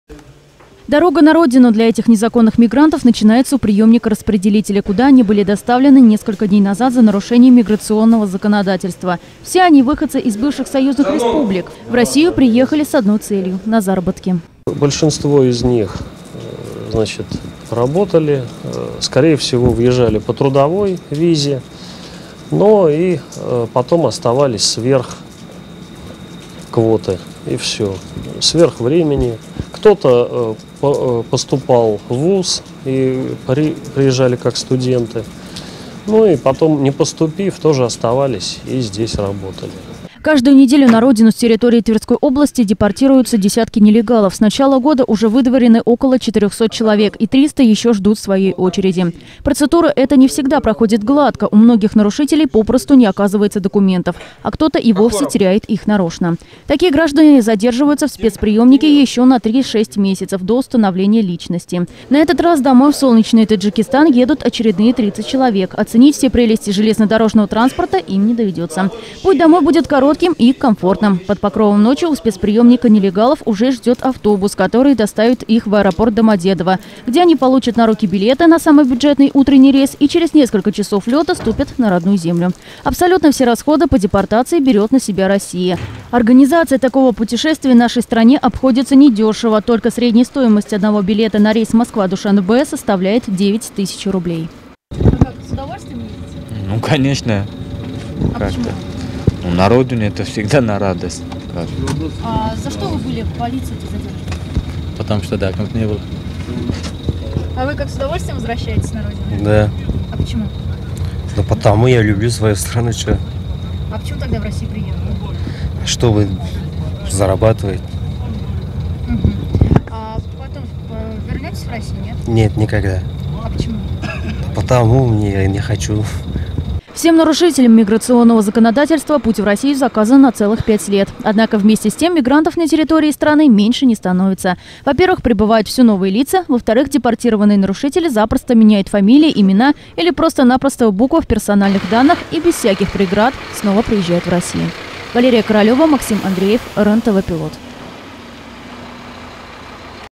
2013-11-07 Родина ждет
В ночь с 6 на 7 ноября из Твери в Душанбе были выдворены 30 нелегальных мигрантов. Дорога в  родные края для них будет быстрой и комфортной -- на самолете, причем бесплатно. Все расходы на себя взяла Россия.